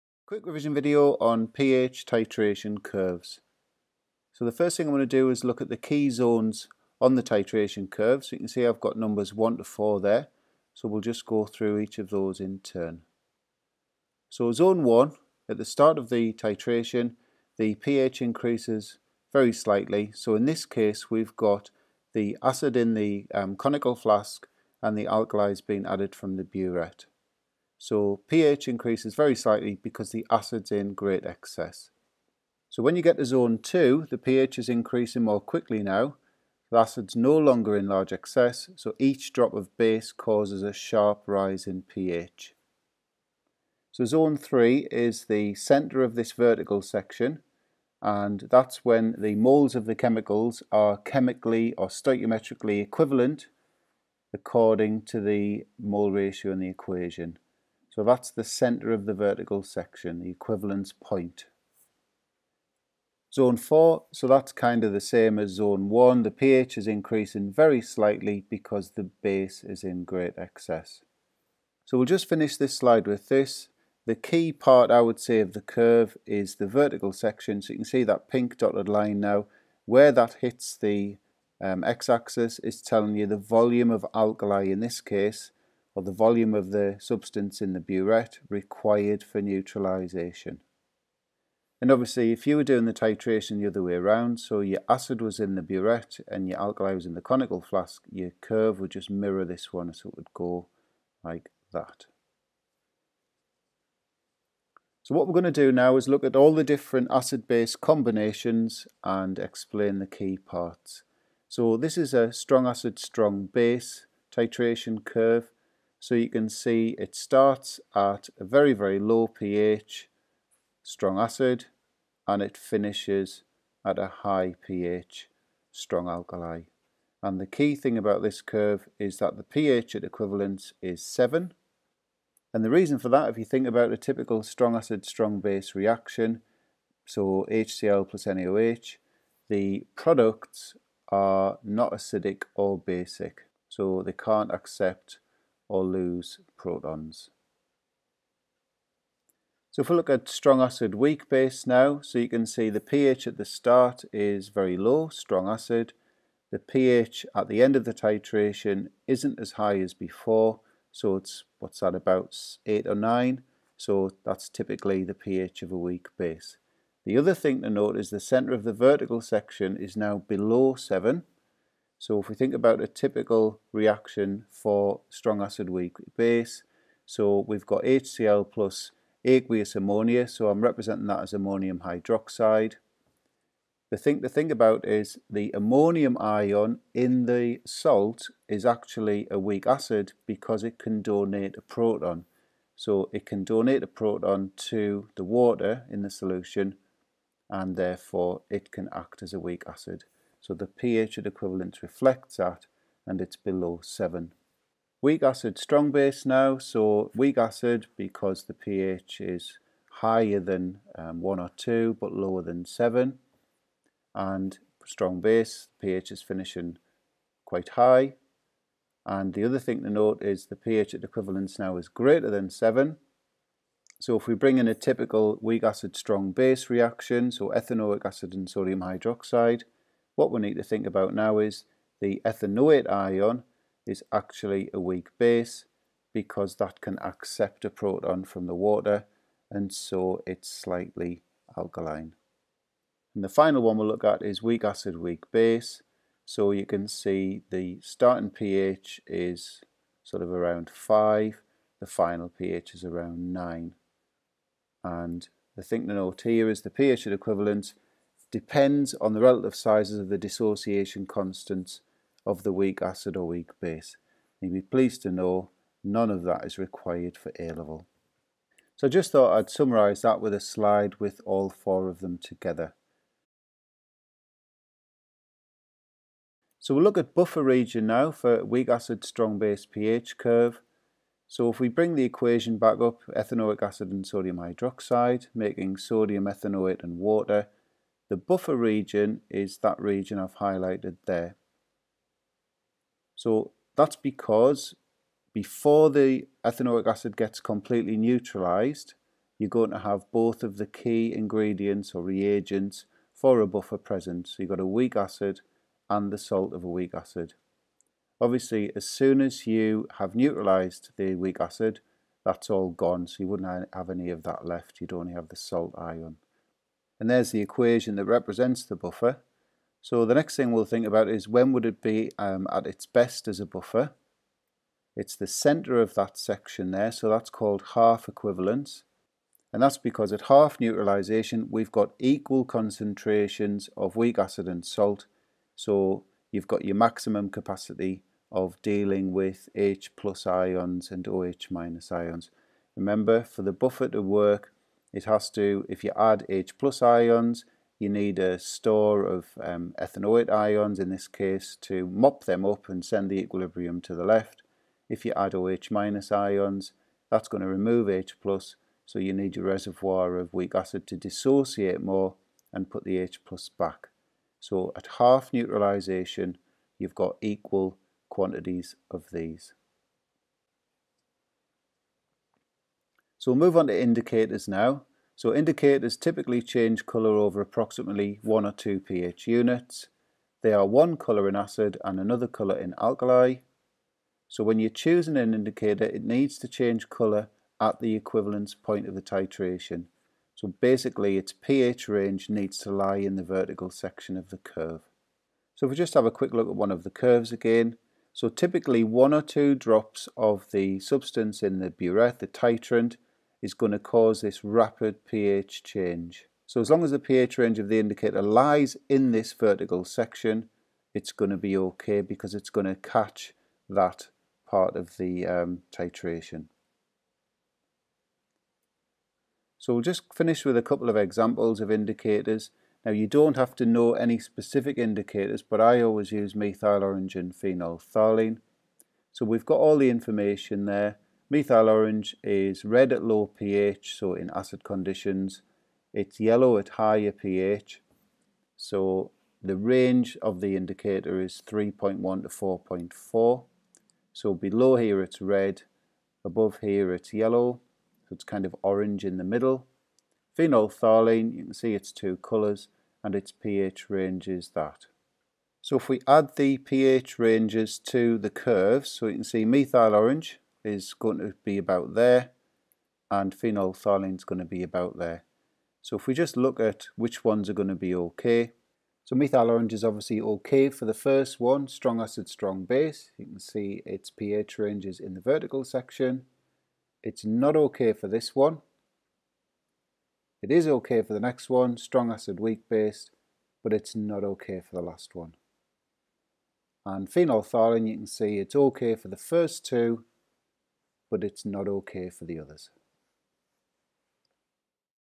Quick Revision - pH titration curves & indicators
Video looks at the key zones of a pH titration curve then goes into more detail for the different acid-base combinations.
pH at equivalence and buffer action also covered.
Video finishes by looking at how to select the correct indicator